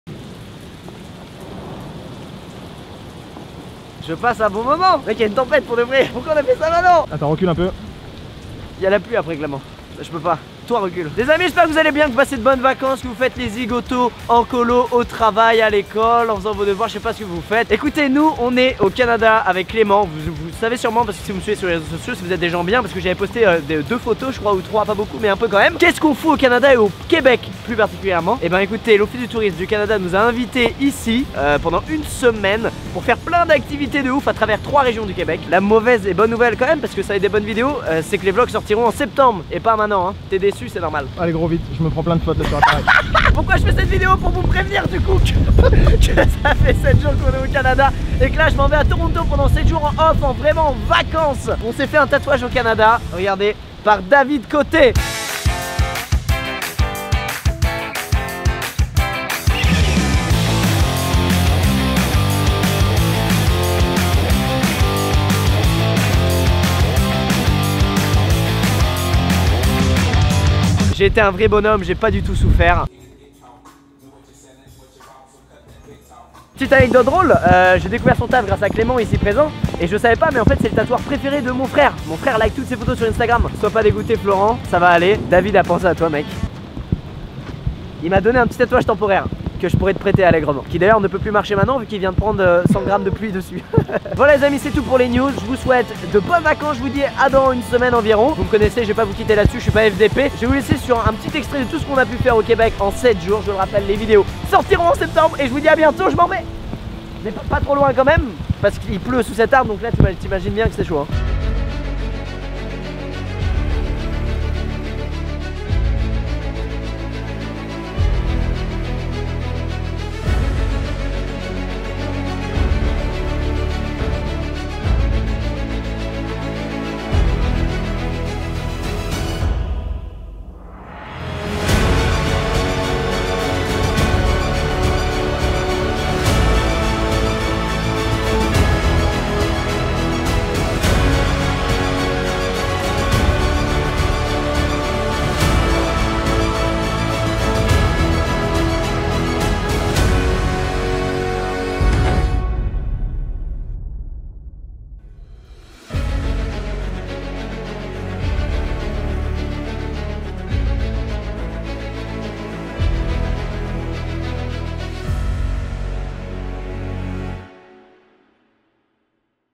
MON TATOUAGE DU CANADA
Après mon tatouage Colombien, Coréen et Parisien : mon tatouage Canadien ! Bonnes vacances tout l'monde :-)